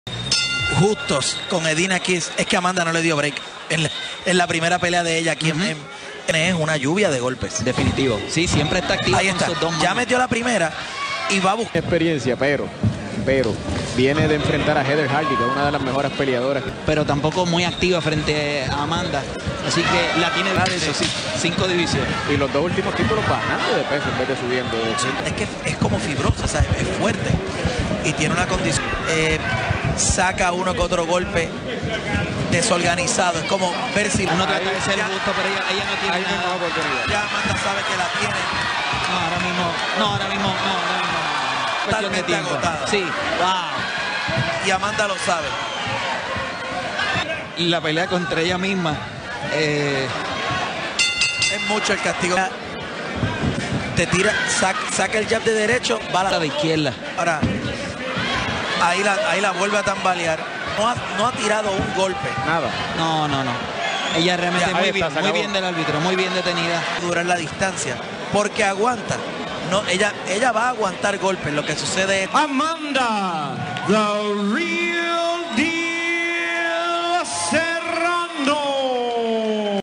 Amanda Serrano Vs Edina Kiss II Highlights - WBO Title
Amanda Serrano (Campeona Mundial WBO) Vs Edina Kiss (Retadora) II - Peso Supergallo - Serrano 2da. Defensa